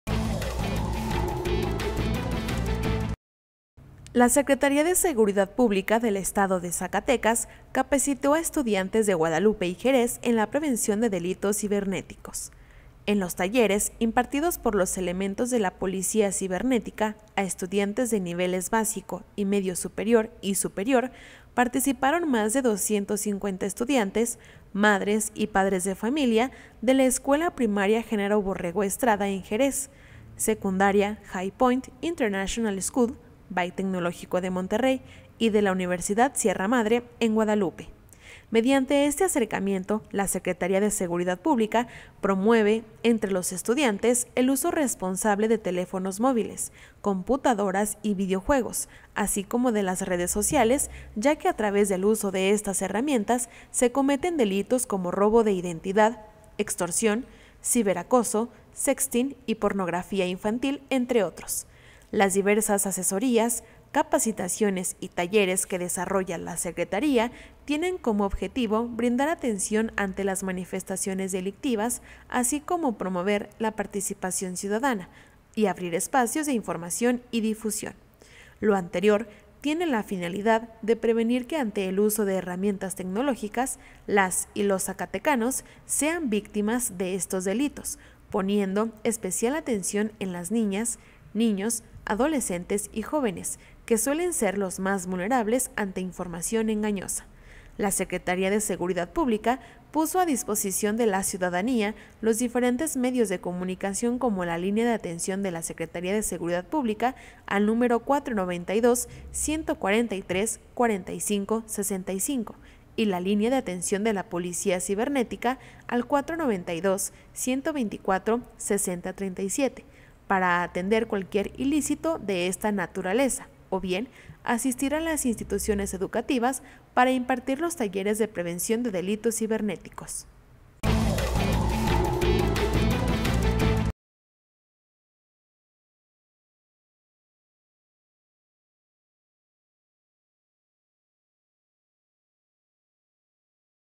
Estudiantes recibieron una capacitación de la SSP para prevenir delitos cibernéticos.
La Secretaría de Seguridad Pública (SSP) del Estado de Zacatecas, capacitó a estudiantes de Guadalupe y Jerez en la prevención de delitos cibernéticos. En los talleres, impartidos por elementos de la Policía Cibernética a estudiantes de niveles básico y medio superior y superior, participaron más de 250 estudiantes, madres y padres de familia de la Escuela Primaria Genaro Borrego Estrada, en Jerez; Secundaria HighPoint Internacional School by Tecnológico de Monterrey y de la Universidad Sierra Madre, en Guadalupe. Mediante este acercamiento, la SSP promueve entre los estudiantes el uso responsable de teléfonos móviles, computadoras y videojuegos, así como de las redes sociales, ya que a través del uso de estas herramientas se cometen delitos como robo de identidad, extorsión, ciberacoso, sexting y pornografía infantil, entre otros.